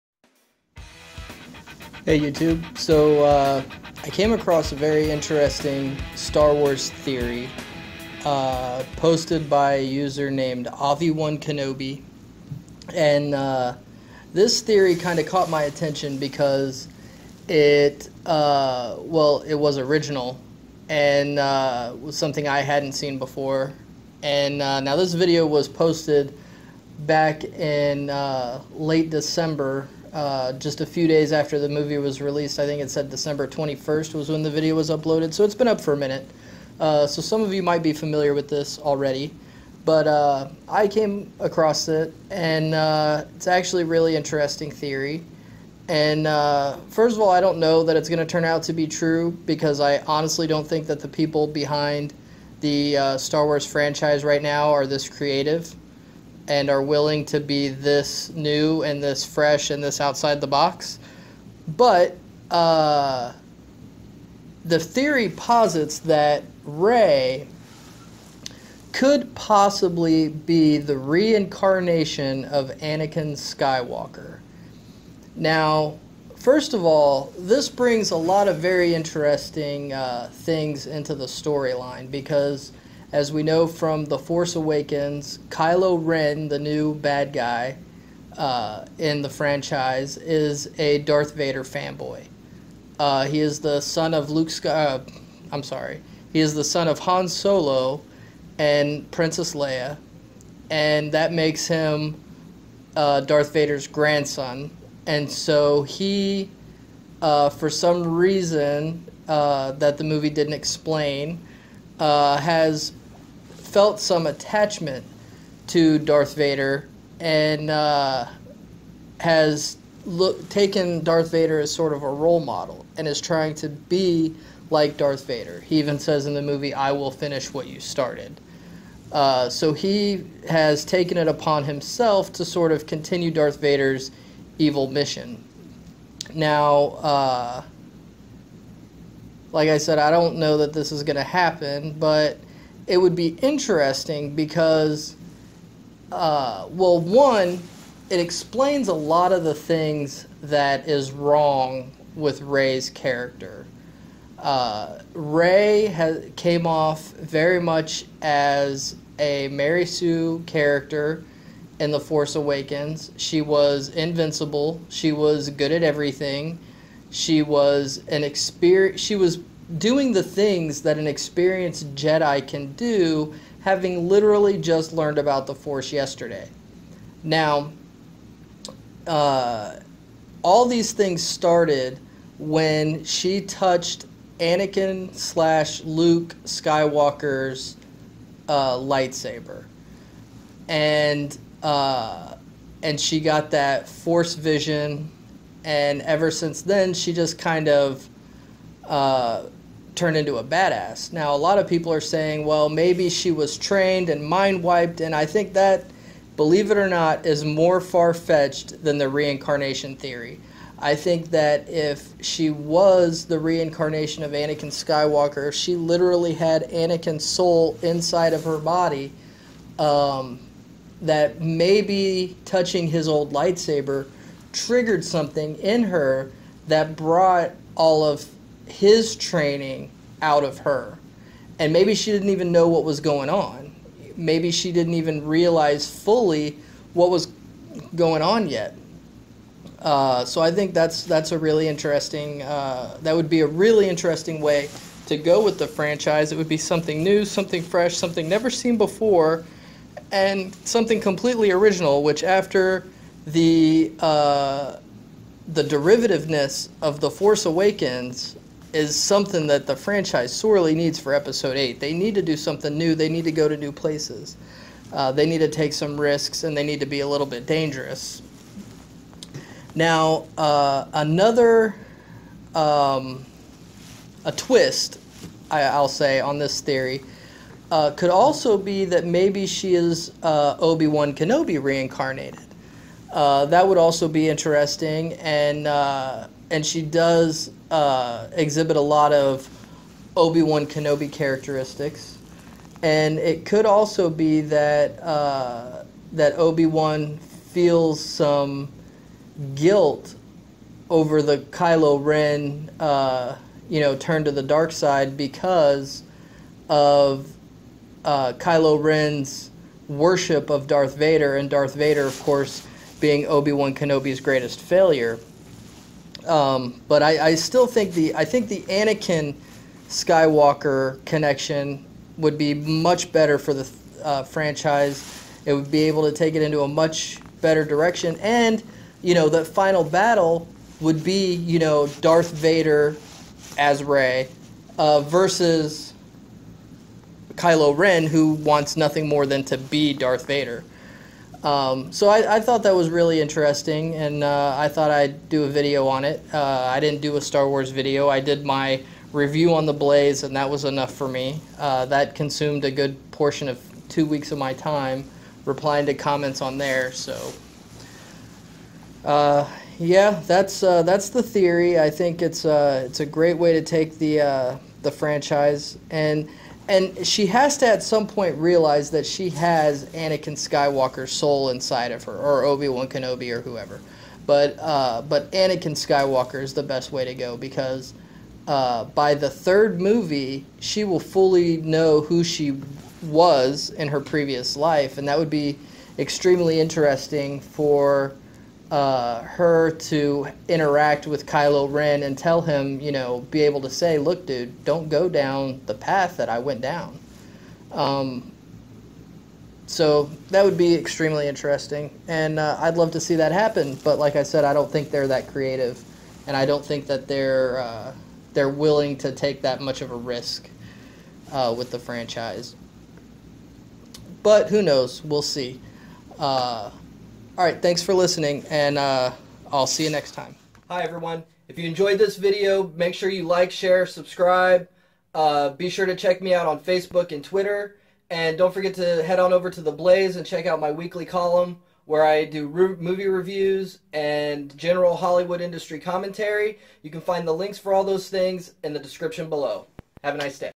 Is Rey Really...? A Star Wars Theory
I got this theory from user Avi-Wan Kenobi.  Be sure to check out his much longer and more detailed video.

My name is William Avitt and I'm the film critic and entertainment columnist for TheBlaze.